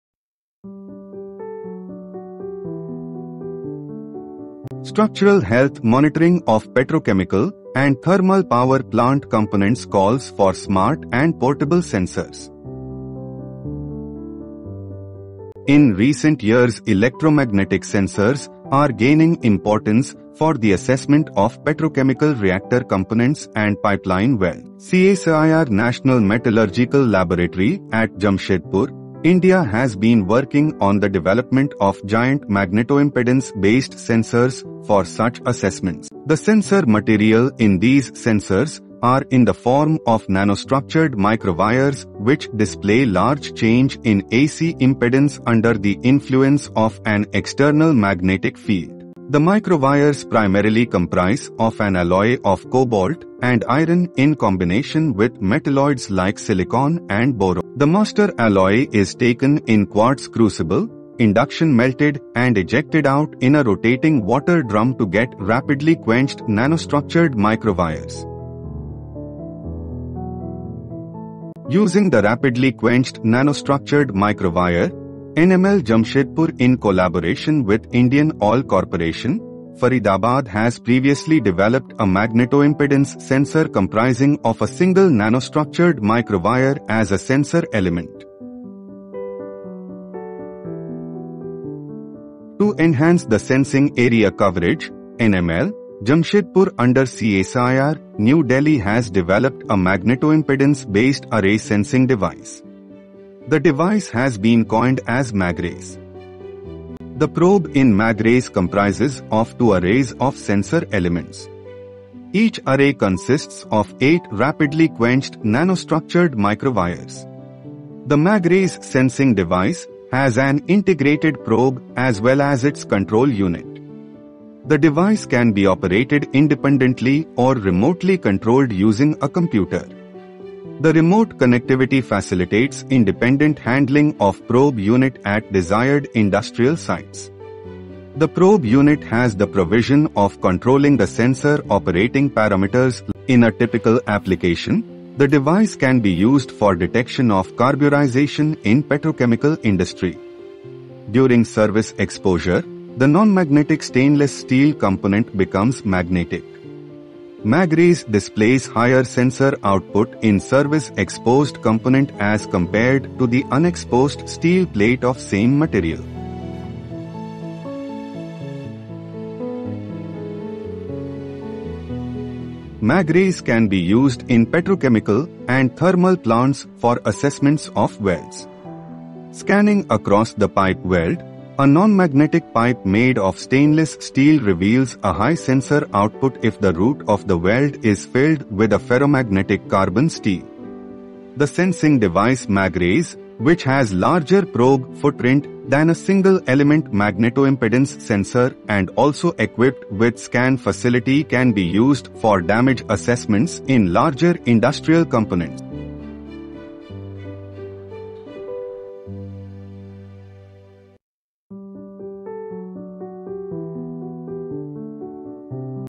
MagRays:Magneto-impedance-based sensing device detects ferromagnetic phase in industrial components
CSIR-National Metallurgical Laboratory, Jamshedpur, India has developed a magnetoimpedance-based array sensing device coined “MagRays”. The device probe comprises an array of rapidly quenched nanostructured wires also prepared in our laboratory which can sense feeble magnetic phases in industrial components. These wires display a large change in magnetoimpedance in the presence of an external magnetic field. The device is lightweight, battery operated, and can be used to scan over the desired area. Thus, the device can be used for structural health monitoring of service exposed components wherein there is a change in their magnetic state.